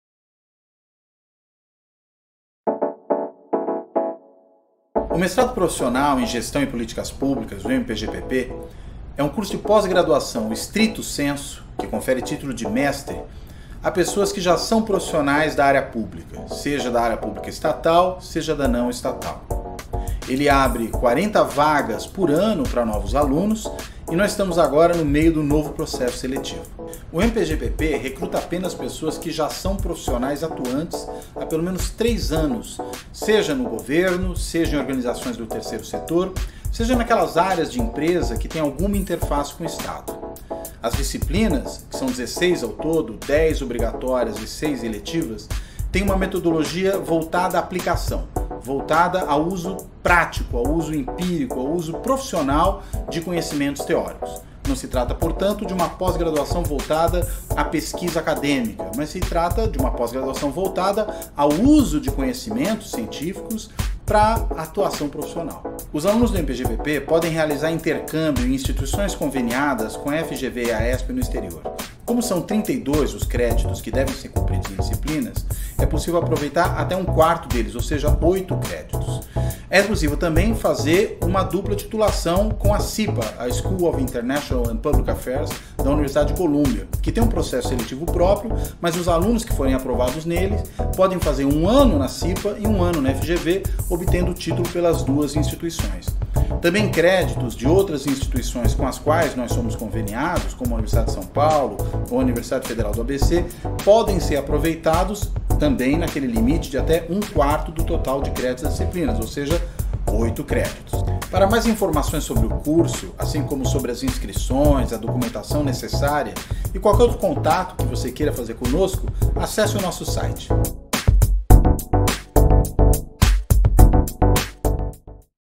Professional Master´s in Management and Public Policy
The MPGPP of FGV’s Sao Paulo School of Business Administration (EAESP) is a program developed for professionals who work in governments and public non-state organizations or in close interaction with the public sector, who seek to develop high-level potential and performance.
In a program conducted in partnership with the world’s renowned Business Schools, students will be able to develop themselves based on the combination of theoretical approaches and applied analysis relating to political science, micro- and macroeconomics, sociology, law, organizational theory, and statistics.
At the end of the program, students will be able to face challenges related to the effectiveness of public policy and quality of public services, as well as contributing to solving public problems, acting as leaders in innovation, with transparency and social commitment.